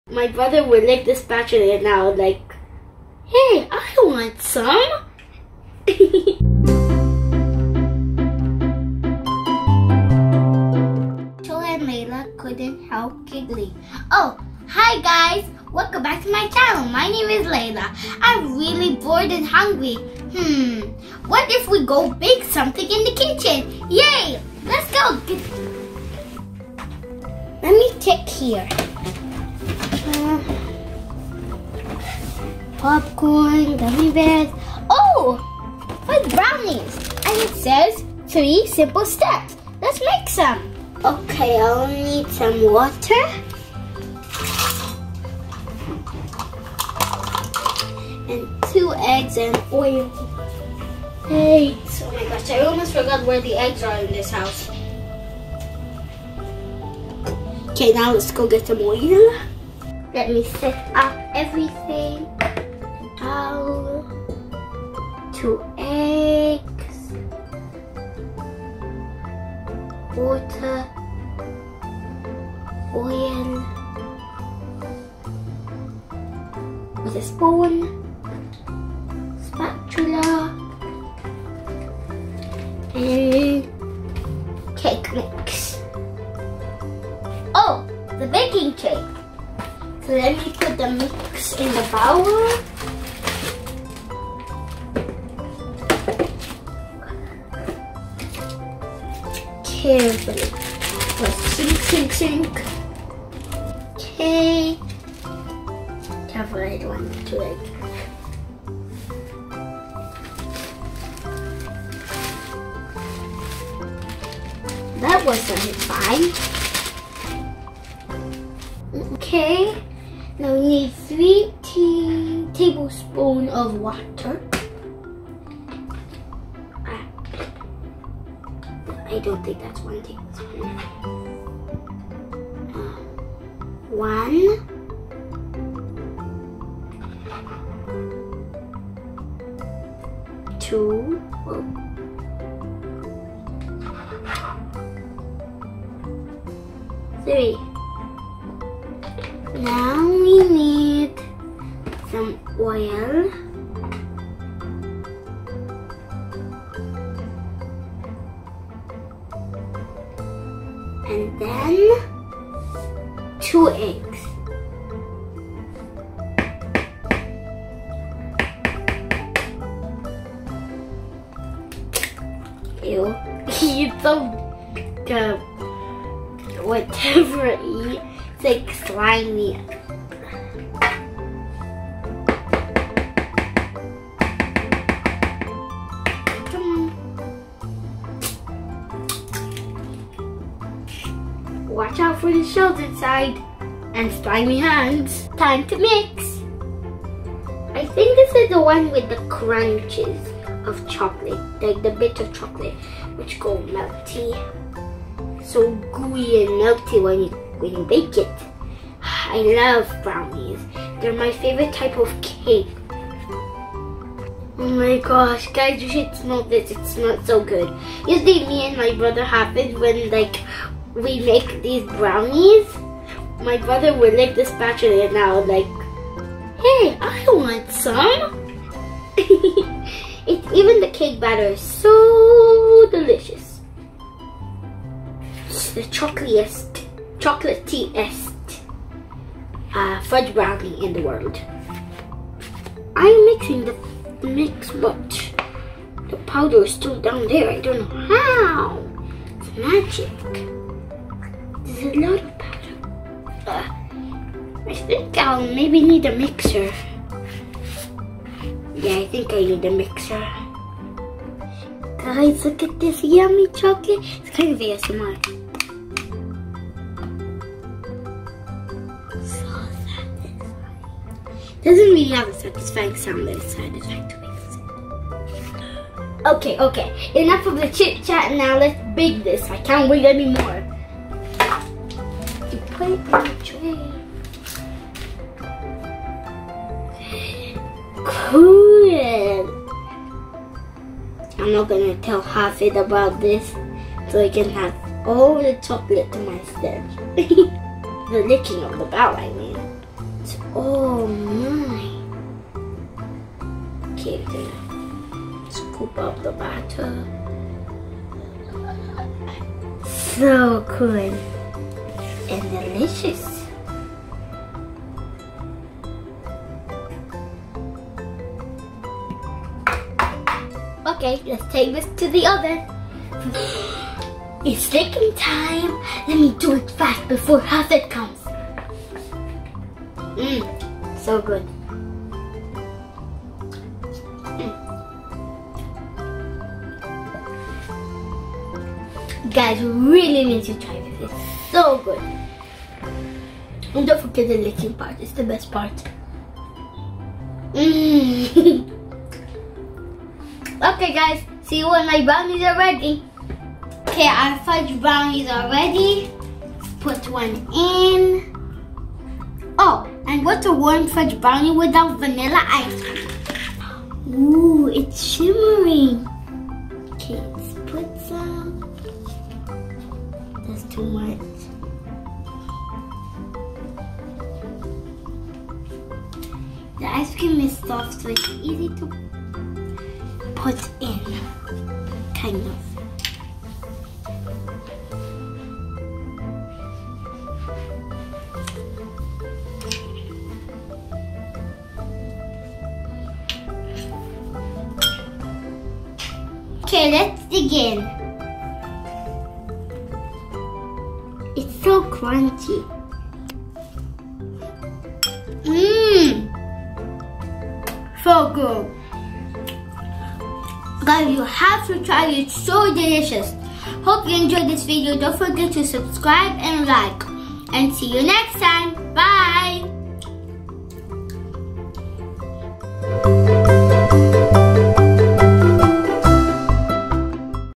Snack Time! Baking Fudge Brownie 🍪| Fun With Layla!
For this delicious Fudge Brownie, you will need:
1 box of Betty Crocker fudge brownie mix
2. Eggs 2 pieces
3. 1/3 cup of oil
4. 3 tablespoons of water
Bake at 350 for 20 to 25 mins.
You must try, its yummy.